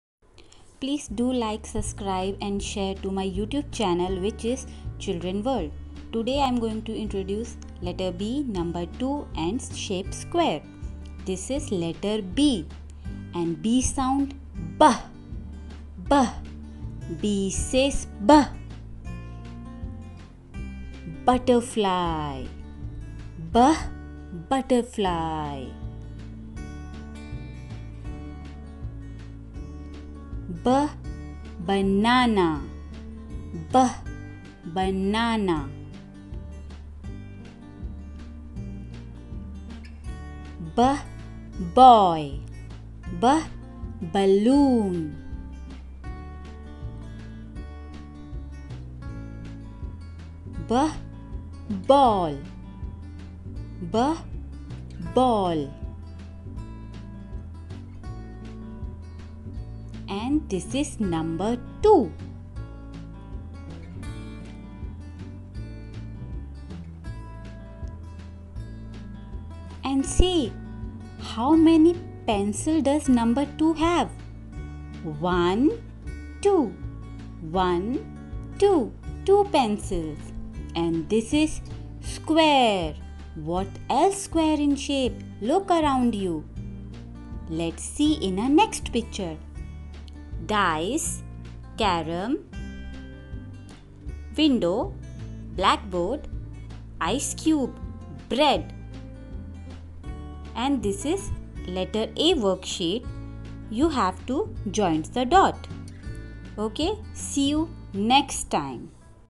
Letter B song /Letter B phonic sound/Letter B vocabulary /Play school at home/fun learning for kids
Today I'm going to introduce   :-                             Letter B                                                                Number 2                                                                   Shape 🔲square. And letter A worksheet slid show for kids so that they can learn and enjoy.